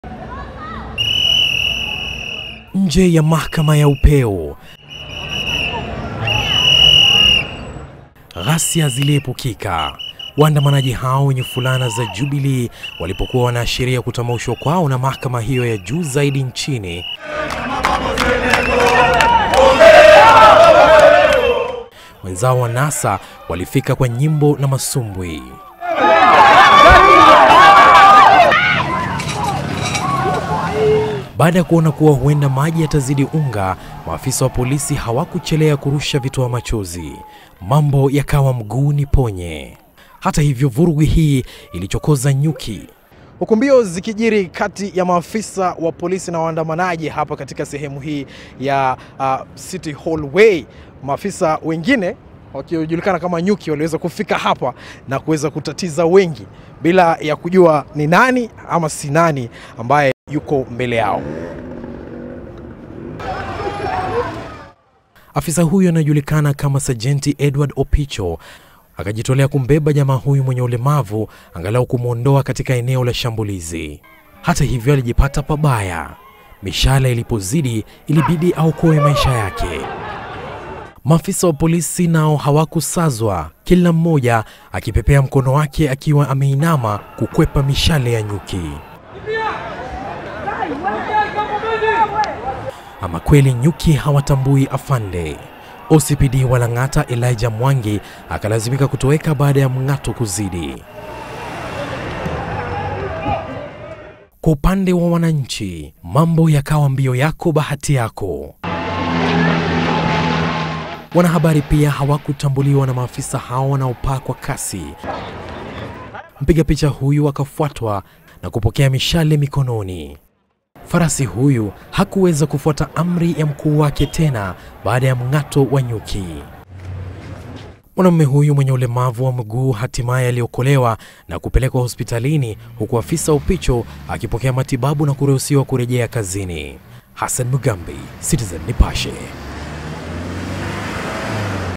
Nyuki wavamia watu nje ya mahakama
Huku majaji wa mahakama ya upeo wakisoma hukumu yao kikamilifu, kizaazaa kilizuka nje ya mahakama hiyo pale ambapo bumba la nyuki liliwavamia waandamanaji, wanahabari na hata maafisa wa polisi baada ya kuchokorwa na waandamanaji waliokuwa wakikwepa ukali wa vitoa machozi walivyorushiwa baada ya kuzua rabsha.

OCPD wa Lang’ata Elija Mwangi, alijipata pabaya baada ya kushambuliwa na wadududu hao wenye hasira.